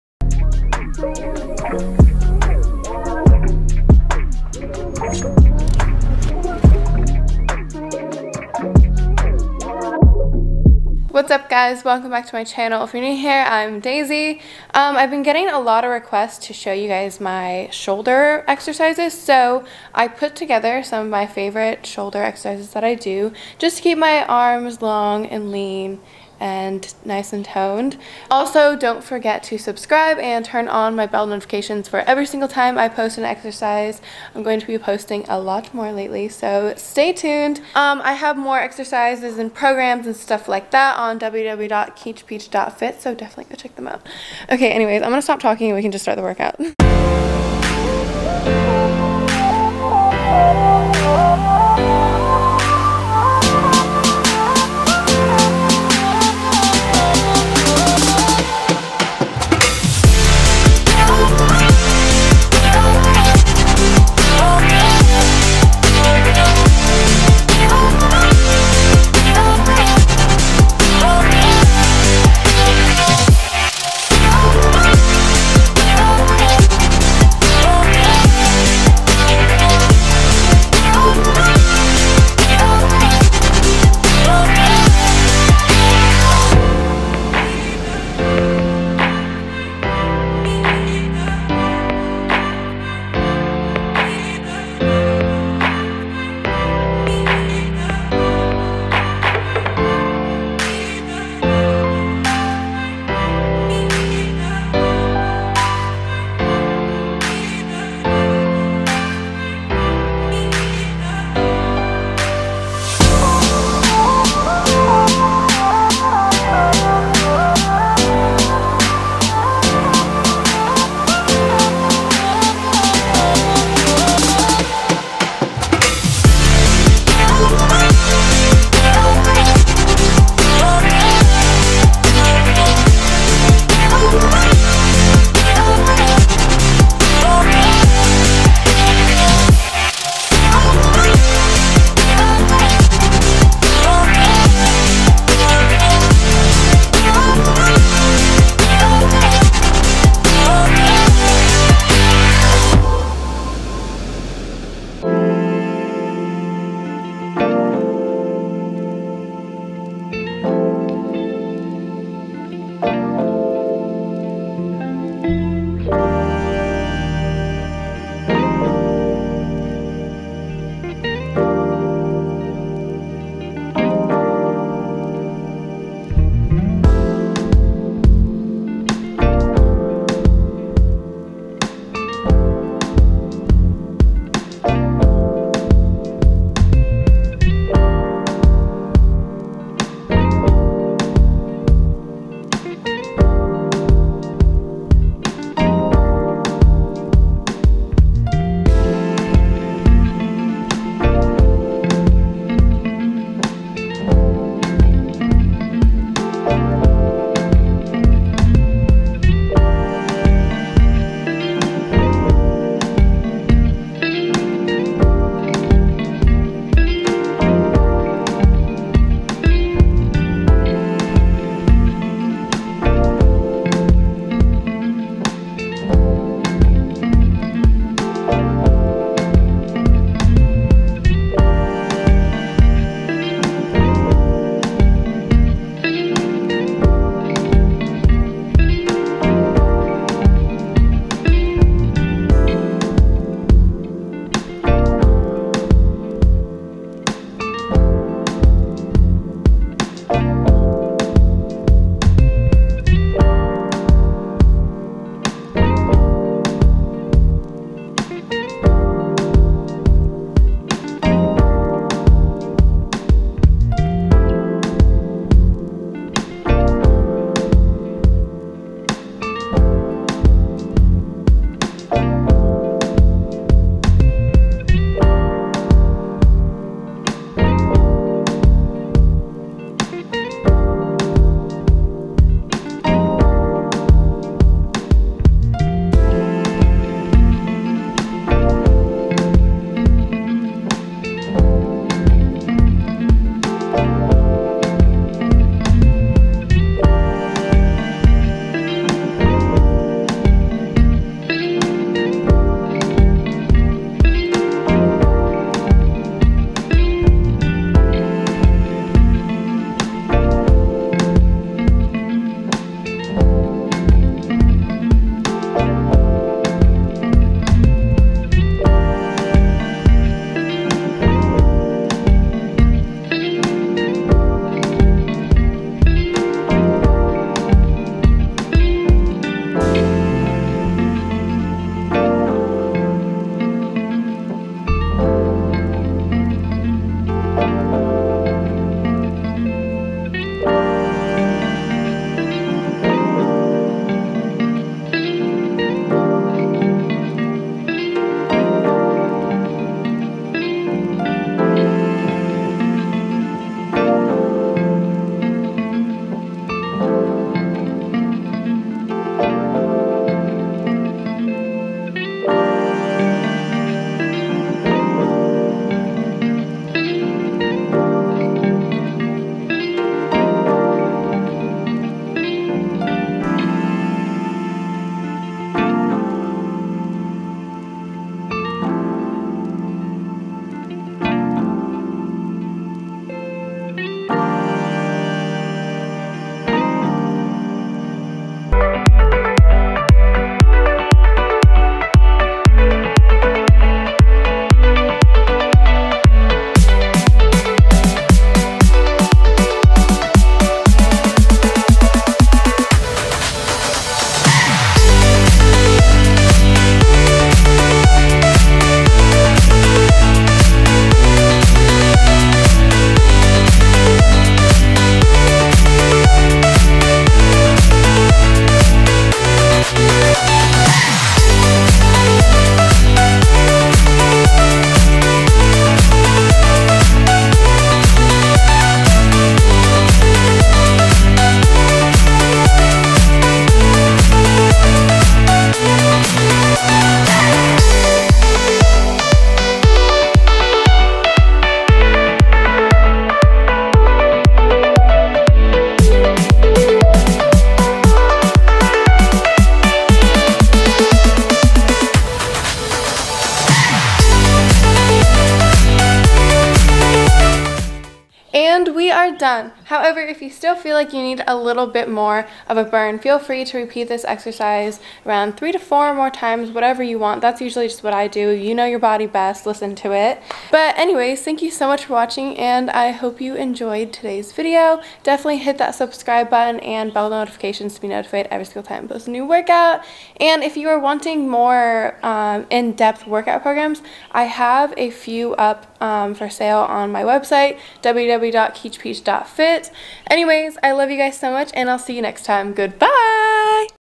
Toned Shoulders Workout | 8 Minutes
Keech Peach Fit | 14 Day Detox 🤍 👇🏻

Hi Peaches!

Here is a quick circuit to keep your shoulders toned and not bulky! Repeat the circuit 3-5 times for more difficulty! Drop a ☀️ in the comments if you are setting goals and working on your summer body!

Comment a workout you would like to see in the comments below!